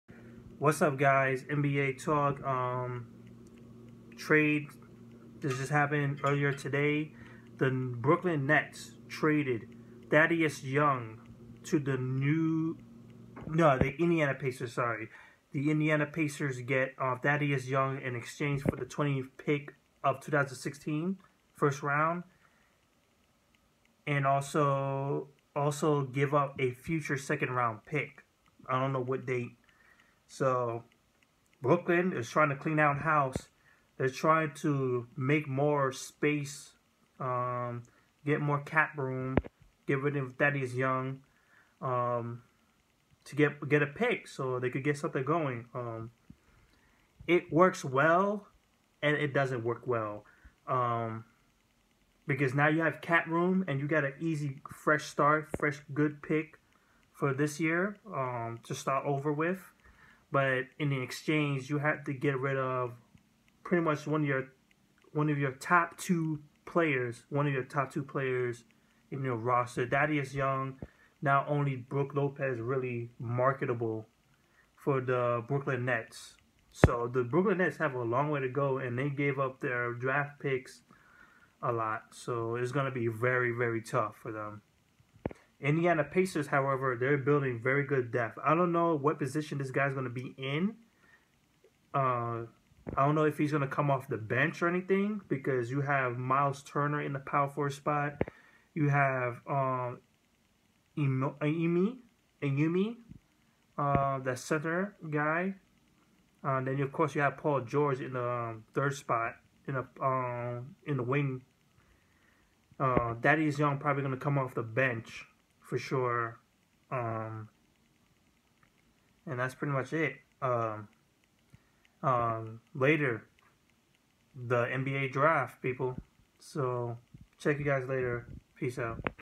Nets- Pacers Trade: Thaddeus Young to Pacers
Brooklyn has traded forward Thad Young to the Indiana Pacers for the 20th pick and a future 2nd rounder